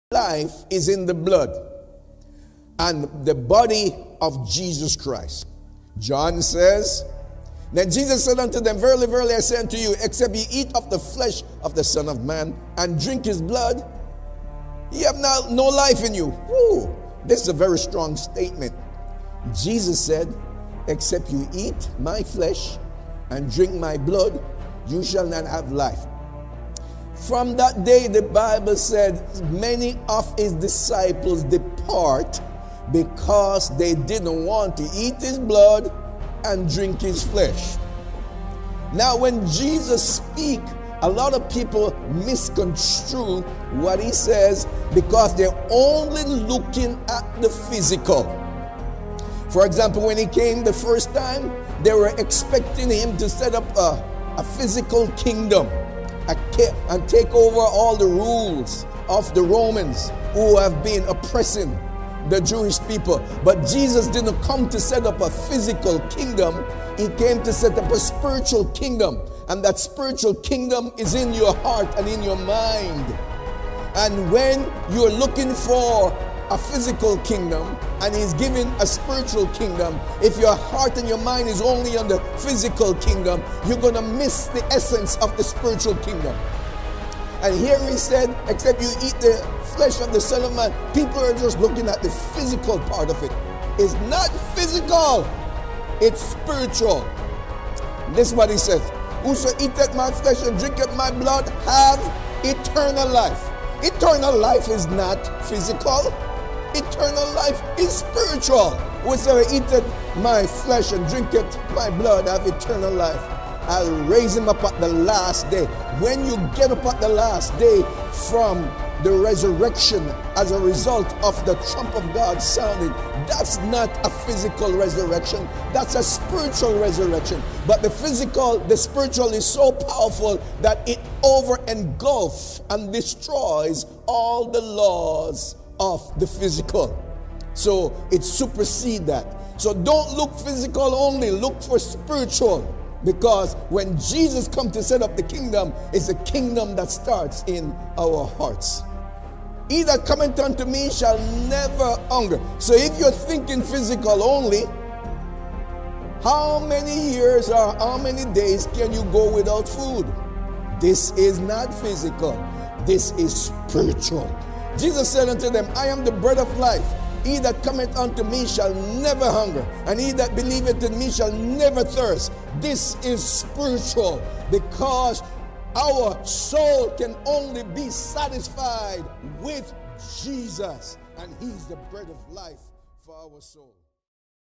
The Spiritual Kingdom Sermonette - Sunday Morning - April 26 2020 - Pastor McEachron
Lighthouse Tabernacle address: 4060 Ridgeway Drive, #1, Mississauga, ON, Canada.